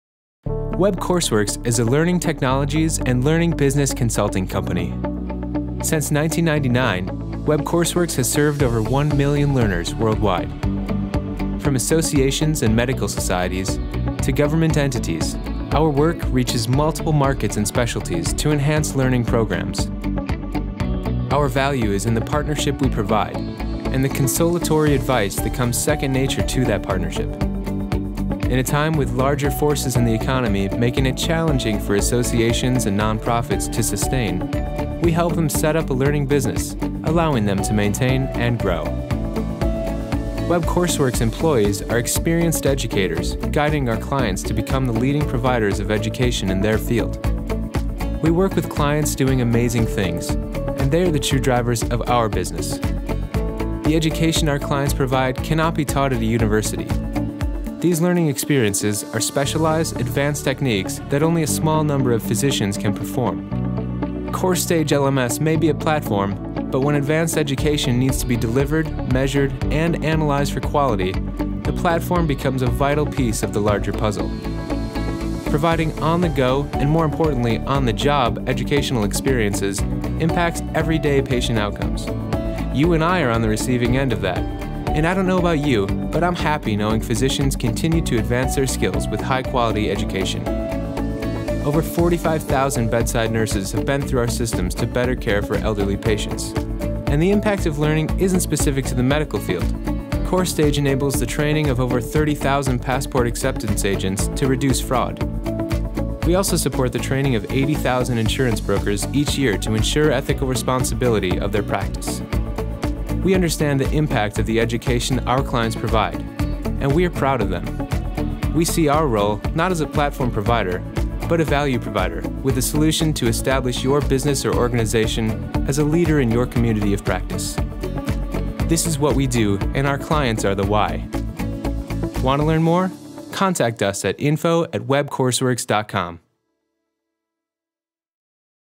Web Courseworks: Your Learning Technology Partner
Web Courseworks is a learning technologies and learning business consulting company. Since 1999, Web Courseworks has served over 1 million learners worldwide. From associations and medical societies to government entities, our work reaches multiple markets and specialties to enhance learning programs. Watch this video to learn more about who we are, the people we serve, and what we do.